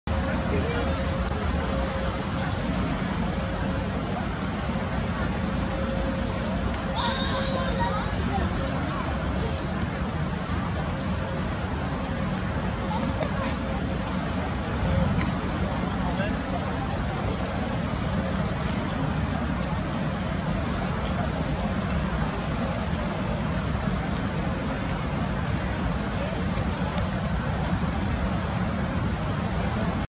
Robin preening
A Robin preens in the Rose Garden.